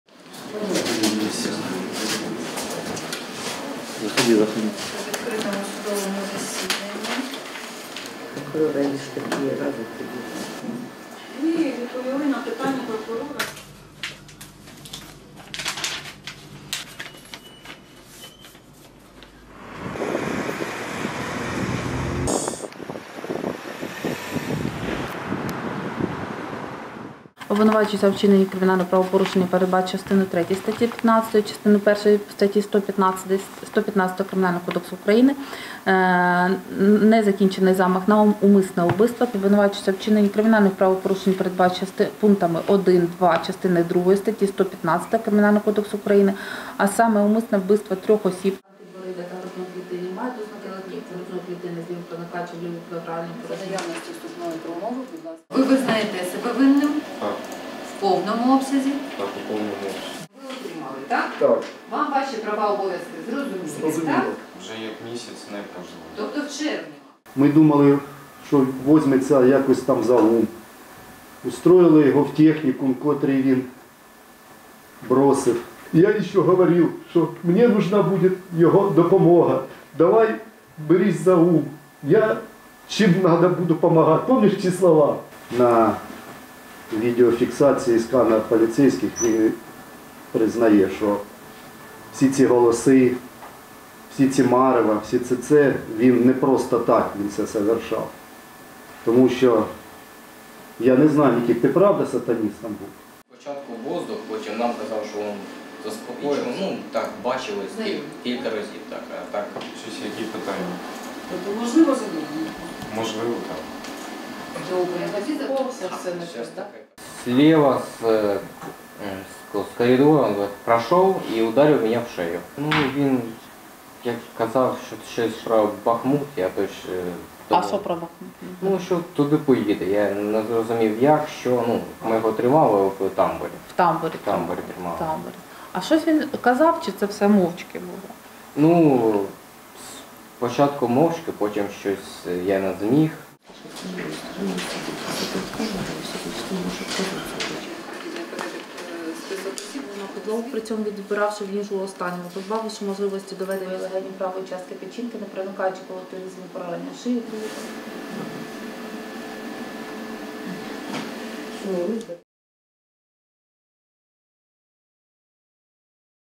Потрійне вбивство родини та смертельне ДТП в Харкові: триває суд над 22-річним підозрюваним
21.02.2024. У Харкові почався суд у справі чоловіка, якого підозрюють у потрійному вбивстві та смертельному наїзді на пішохода під час спроби втекти з місця злочину. Це сталося влітку 2023 року, після затримання чоловік казав слідчим, що скоїв вбивства за наказом Люцифера, розповідав тоді начальник харківської поліції Володимир Тимошко. Сьогодні свідчити на суді обвинувачений відмовився.

Репортерка: Валерія Ємець
Оператор: Антон Каліта
Режисер монтажу: Олександр Коновалов